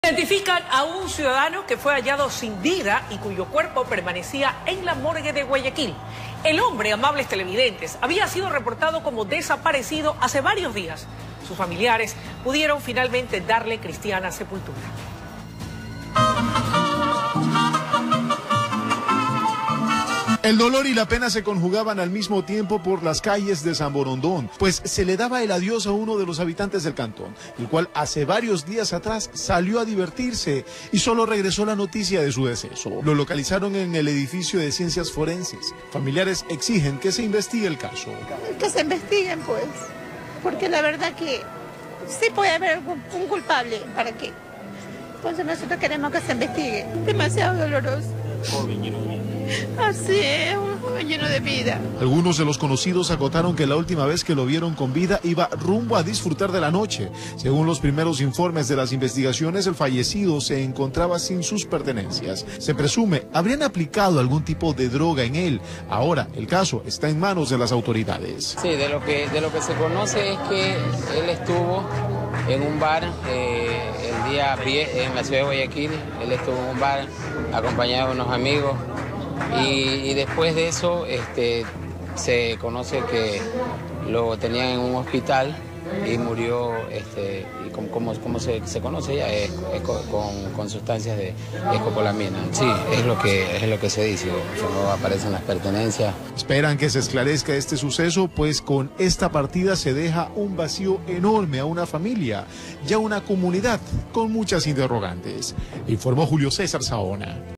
Identifican a un ciudadano que permanecía en la morgue de Guayaquil
La víctima se encontraba reportado como desaparecido desde hace días

Sus familiares y amigos pudieron darle finalmente cristiana sepultura. El hombre vivía en un sector de las calles Samborondón.